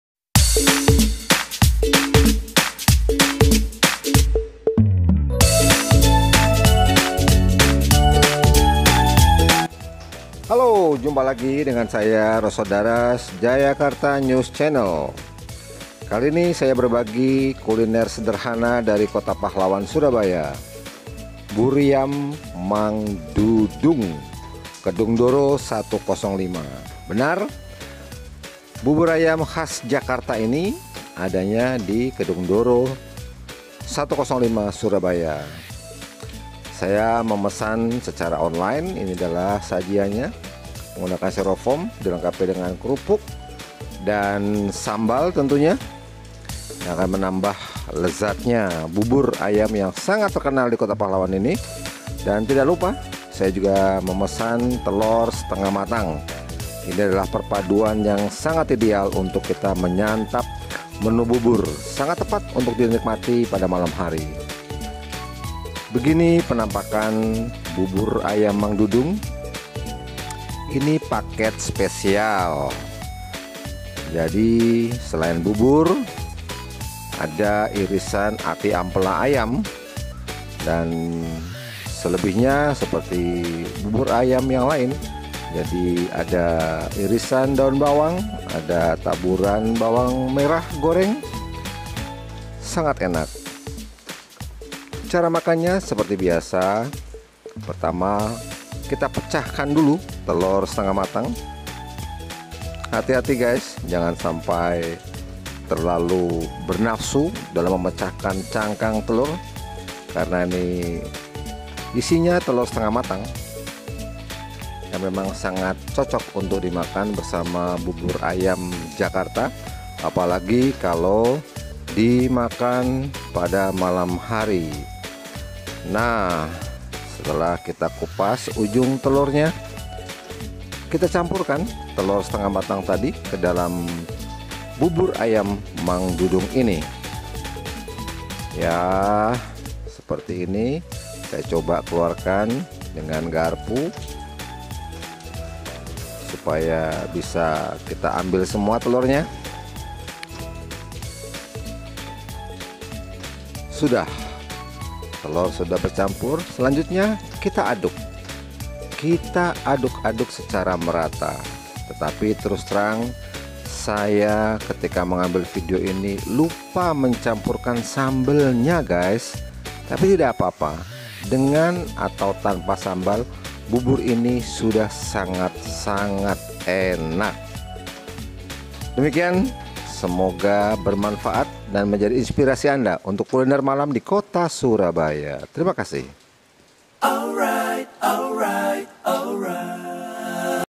Buryam Mang Dudung
Di Surabaya, ternyata ada bubur ayam Jakarta yang sangat terkenal: Buryam Mang Dudung, Kedungdoro 105, Surabaya. Bubur ayam ini menjadi buruan masyarakat Kota Pahlawan, utamanya di malam hari.

Dengan harga berkisar antara Rp 12.000 sampai Rp 18.000, cukup murah-meriah, dan ramah bagi kantong mahasiswa.